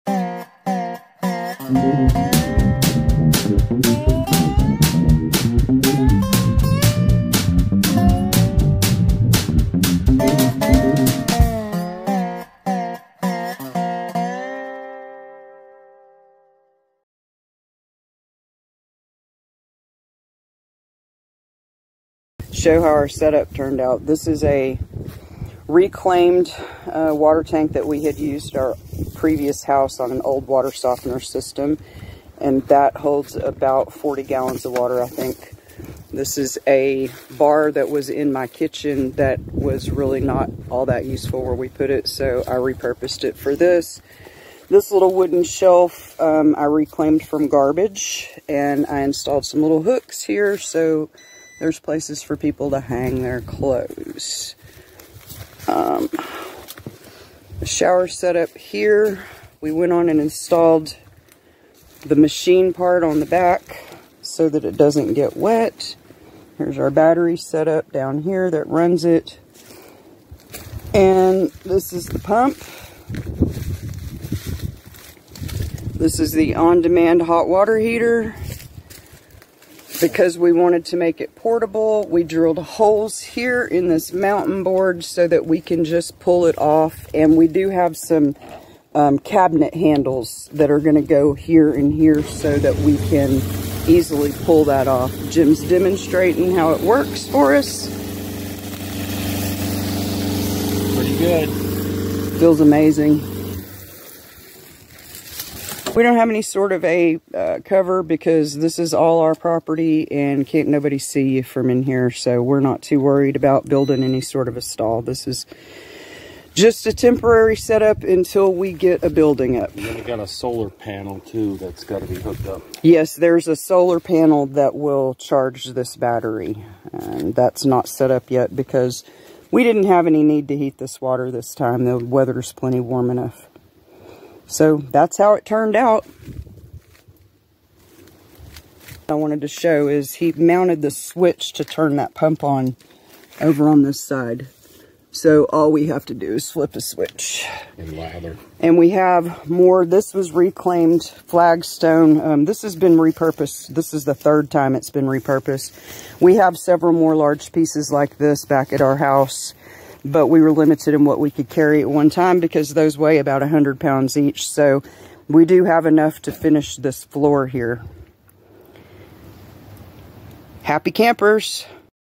Off-Grid Portable Outdoor Shower Build
Showing our build and setup of an off-grid portable shower. It uses an on demand hot water heater that is mounted onto a removable board, so that it can be stored inside away from the elements when not in use. This was a husband and wife DIY project. He is the engineer and I am the designer. He loved my idea of sawing holes to hang the board on hooks and adding the "cabinet" handles (actually gate handles for Jim's bigger hands), a very simple solution for which he already had all the tools. The 2×4 that the board hooks onto is treated lumber leftover from a previous project. The plywood the heater is mounted on was cut down from a new half sheet we bought at the hardware store. The mounting and clothes hooks were all we had to buy besides low pressure regulator, on demand heater, and pump. Everything else was repurposed.

If you have this kind of heater (18L Tankless Outdoor Hot Water Heater) and can't get it to work:
***It requires a 1 lb max low pressure gas regulator.***
Order of operations is shown in this video Thanks to the creator @jtflovin for getting us lined out!

YOU MUST USE THE 1 LB REGULATOR. It won't work with anything higher. You will just burn out your solenoid trying.